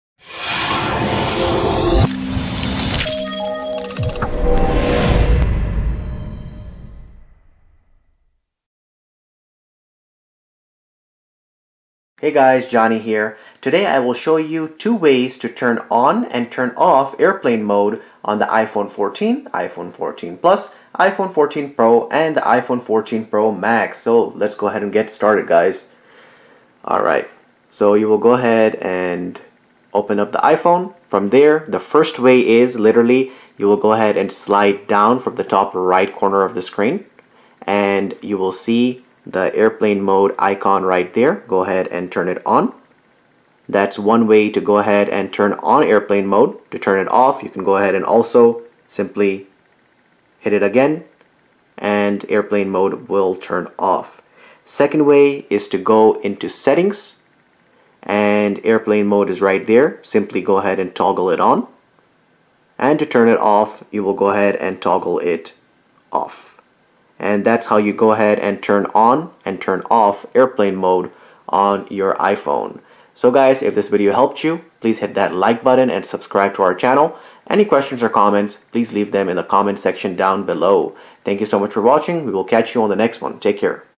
How To Turn ON/OFF Airplane Mode (Enable/Disable): iPhone 14/14 Plus/iPhone 14 Pro/iPhone 14 Pro Max
- In this video I will show you how to turn on/off airplane mode on the iPhone 14/iPhone 14 Plus/iPhone 14 Pro/iPhone 14 Pro Max.